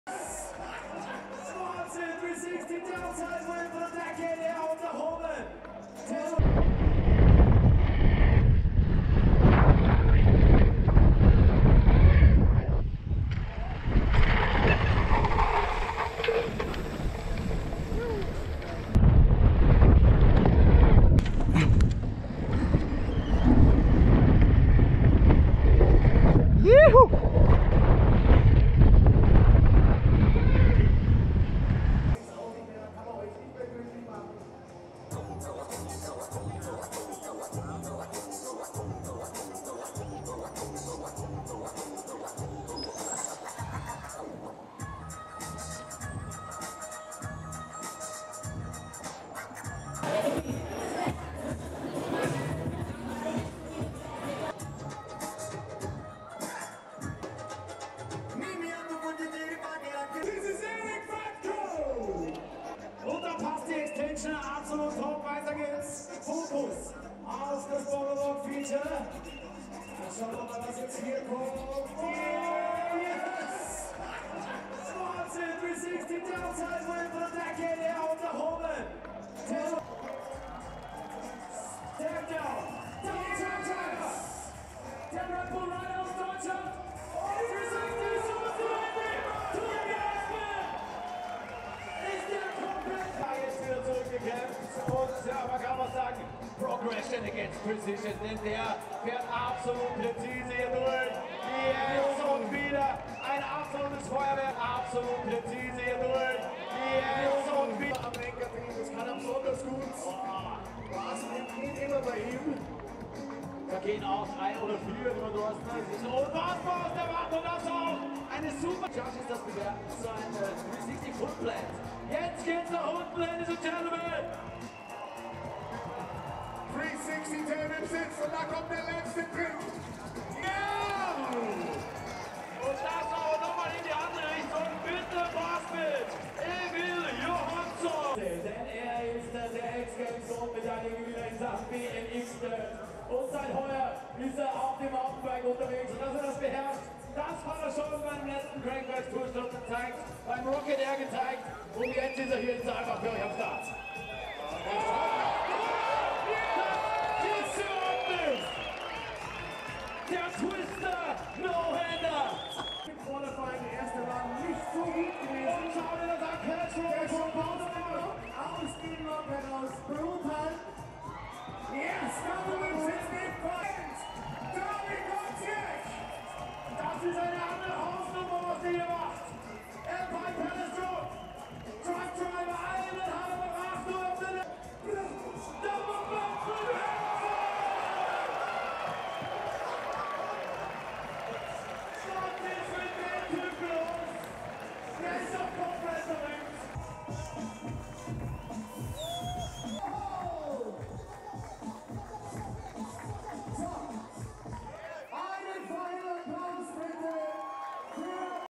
GlemmRide Bike Festival 2019 - Slopestyle
Per il quinto anno consecutivo, Saalbach Hinterglemm invita a un raduno festivo: tra il 3 e il 7 luglio 2019, la bici Eldorado ospiterà nuovamente il GlemmRide Bike Festival. Le star internazionali della scena della mountain bike si riuniranno e mostreranno alcuni trucchi sorprendenti. Quest'anno il Slopestyle Contest è classificato come un evento d'oro del Freeride Mountain Bike World Tour (FMB). Il percorso urbano sul leggendario pendio di Goaßstall e sui tetti dell'Hotel Alpine Palace è il cuore del festival. Dopo la partenza al famoso rifugio Après-Ski Goaßstall, i ciclisti raggiungono una serie di enormi salti e ostacoli, tra cui una falla sul fiume Saalbach, fino alla zona del festival nel centro di Hinterglemm.